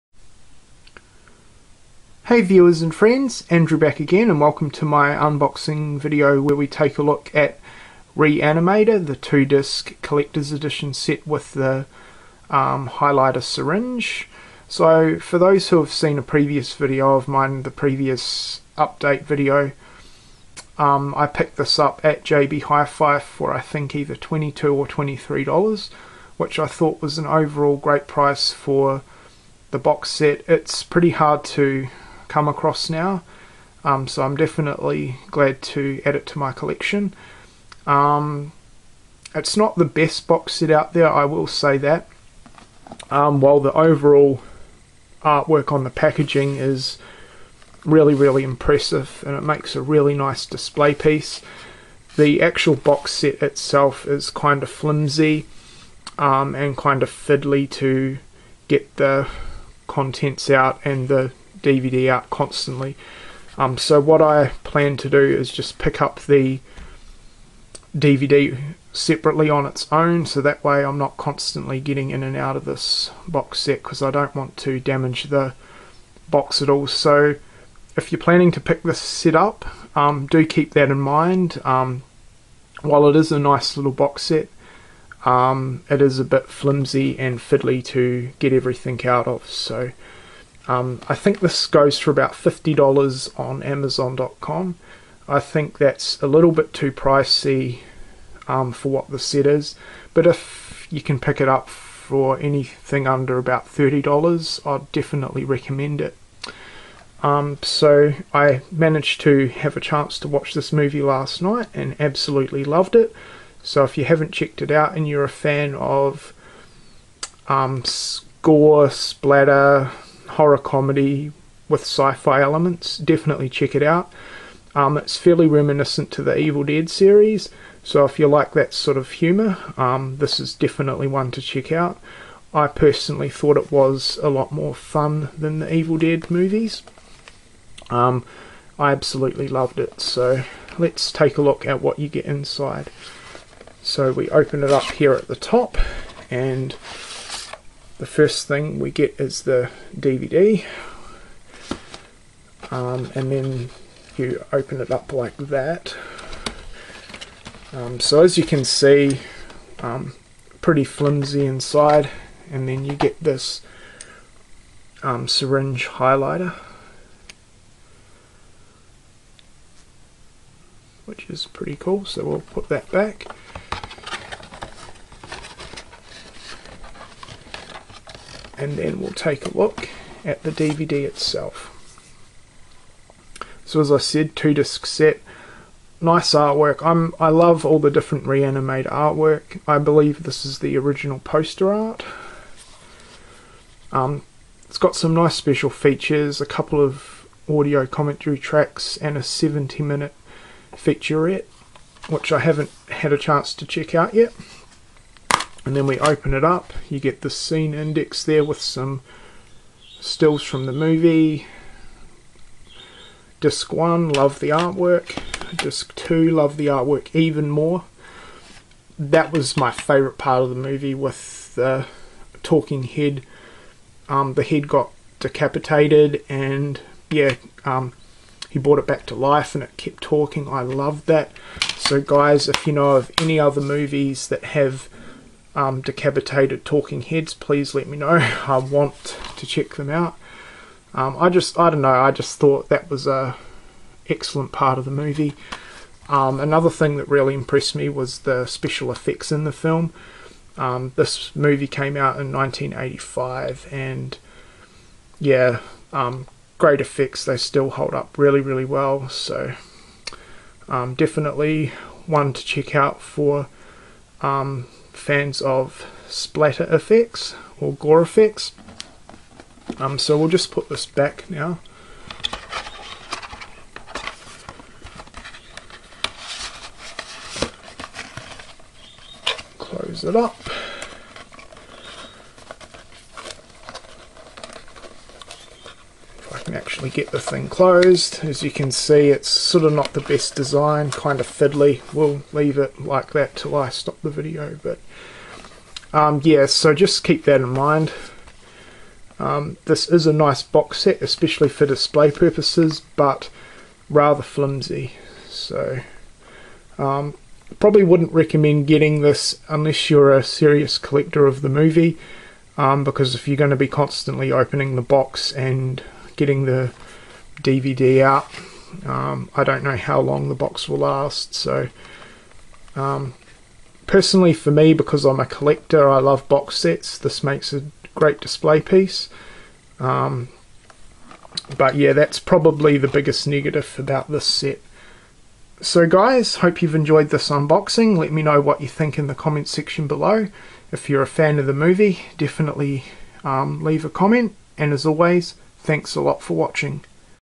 Re-Animator Boxset
Here's an unboxing of the Re-Animator Collectors Edition Boxset.